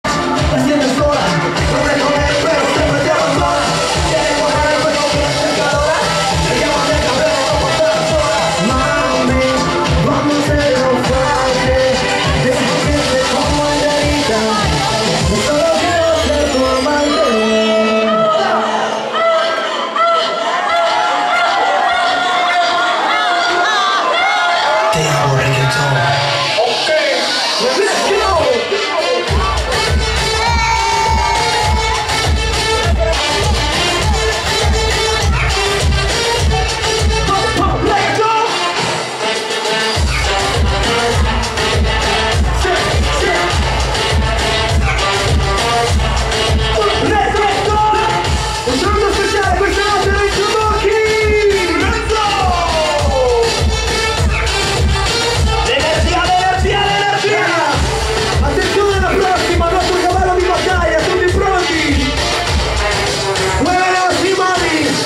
Venerdi 31 Maggio 2019 La Grande Chiusura del Mad'in Italy Verona
La Prima del Nuovo Format Te Amo Reggaeton La Serata Dedicata alle Donne Sismica Sul Palco Somos Amantes